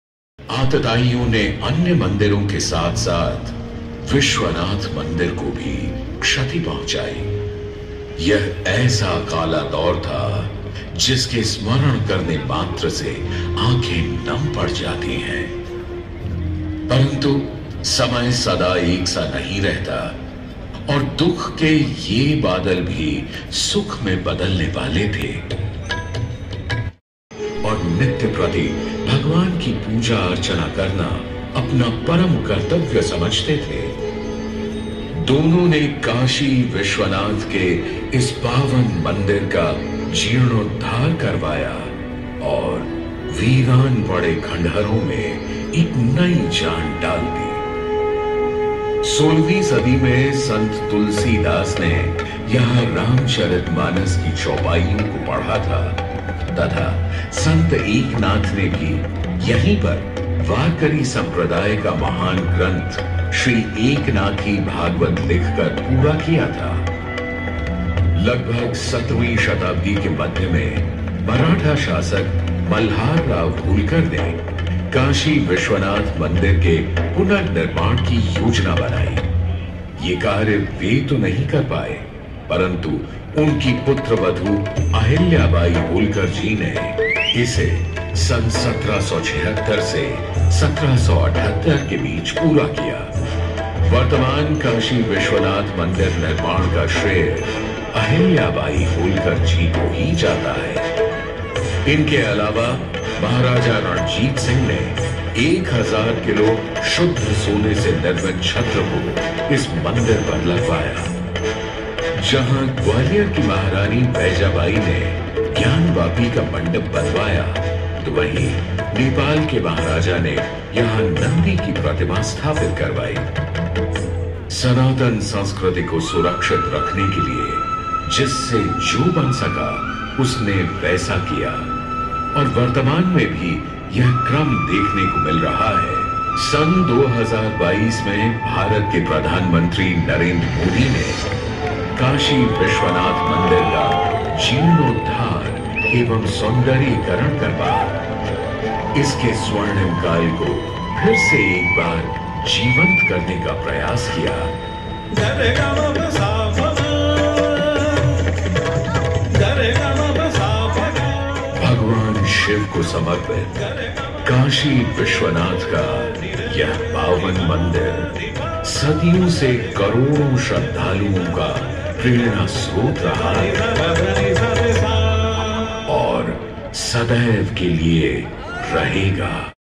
A shortfilm of Kashi Vishwanath| आक्रांताओं के द्वारा तोड़ा गया काशी विश्वनाथ मंदिर।
A short film of Kashi Vishwanath| आक्रांताओं के द्वारा तोड़ा गया काशी विश्वनाथ मंदिर। कई राजाओं महाराजाओं के द्वारा मंदिर को जीर्णोद्धार करवाया गया आखिरकार काशी विश्वनाथ मंदिर को भारत के प्रधानमंत्री नरेंद्र मोदी के द्वारा जीर्णोद्धार कर कॉरिडोर बनाया गया